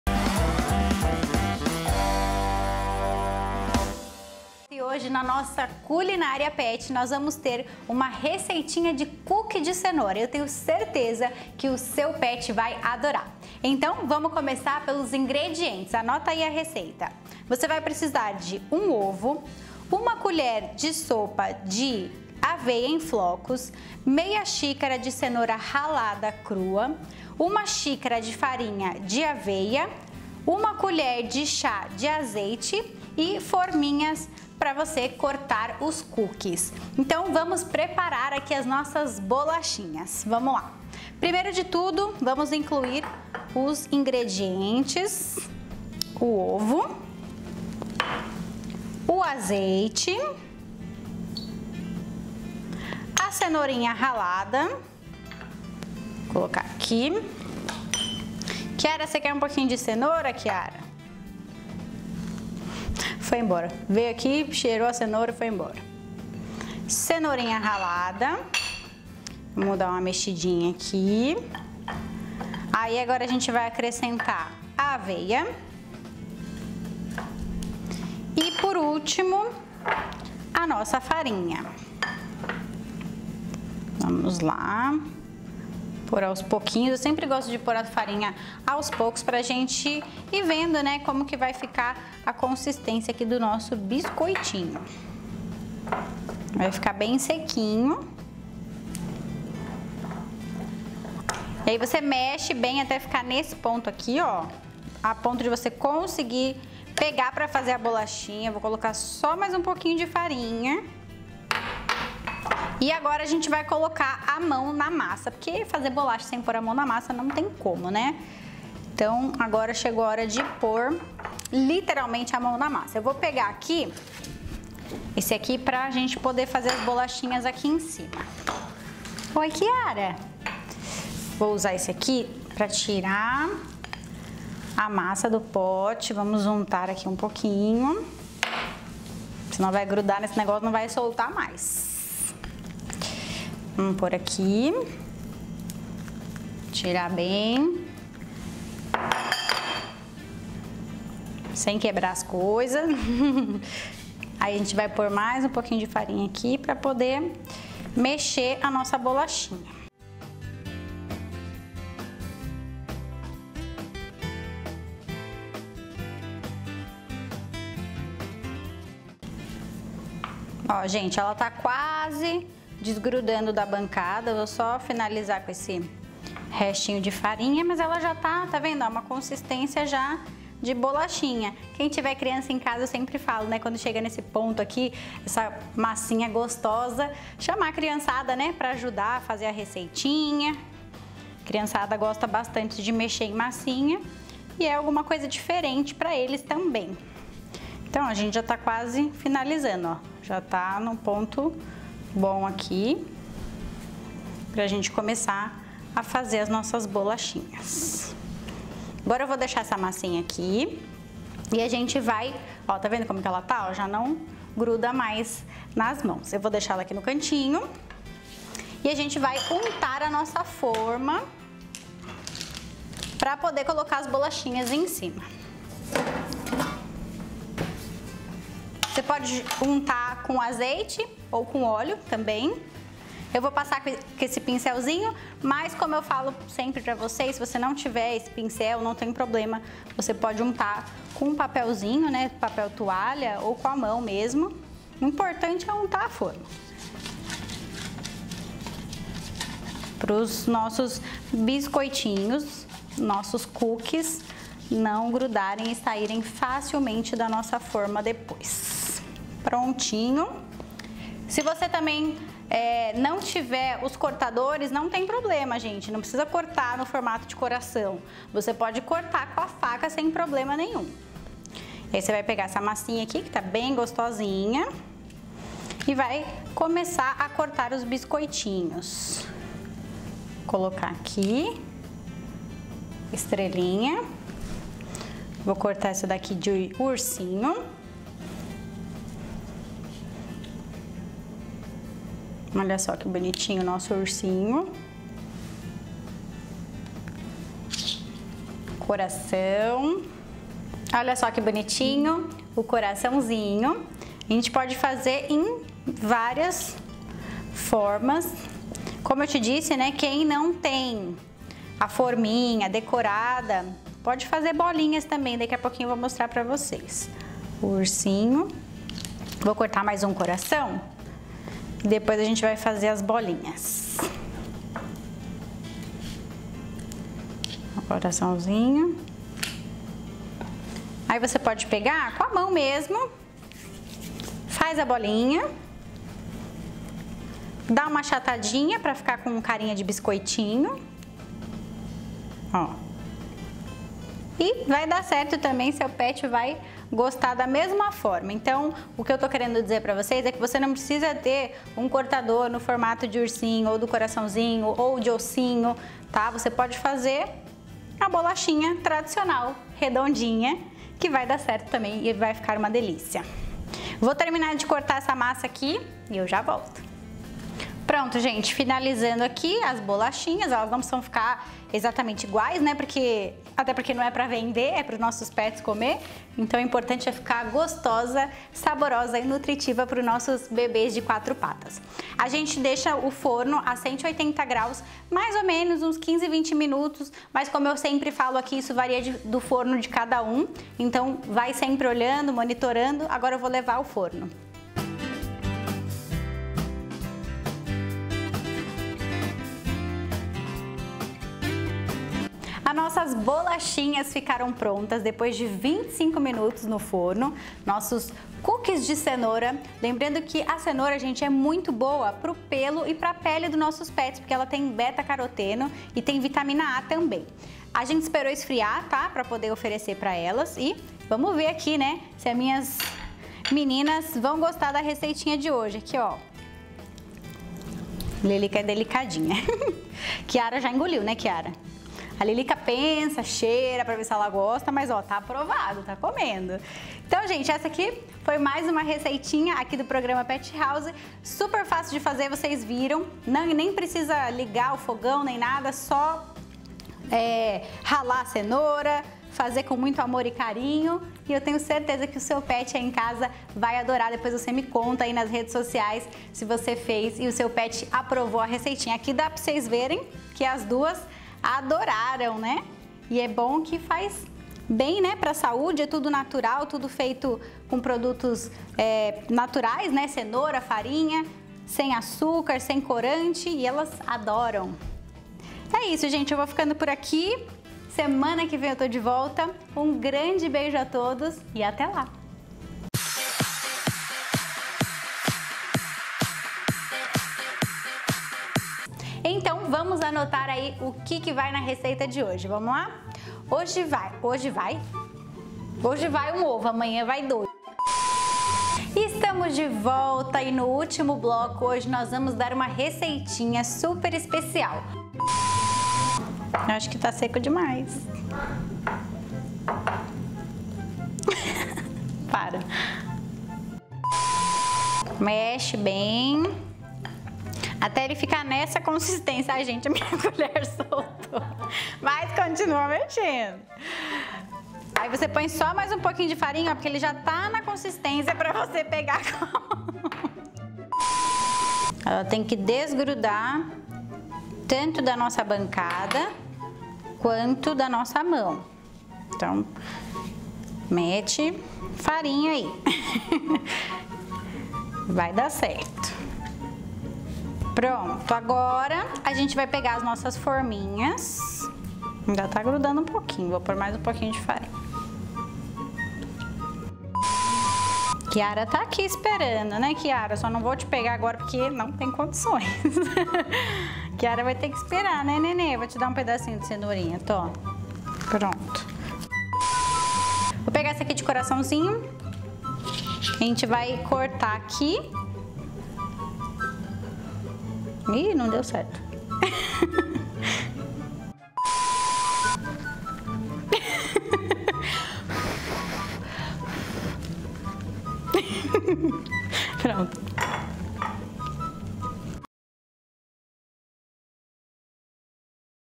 COOKIE DE CENOURA -Receitinha Pet House
No vídeo de hoje vou ensinar a fazer "Cookie de Cenoura" para os pets! 🐶🐱
Uma receita super fácil e nutritiva para os nossos filhos de patas! ❤️🐾
Tenho certeza que o seu pet vai adorar esse receitinha deliciosa!

*Assista o vídeo até o final, tem uma surpresinha da edição! 😉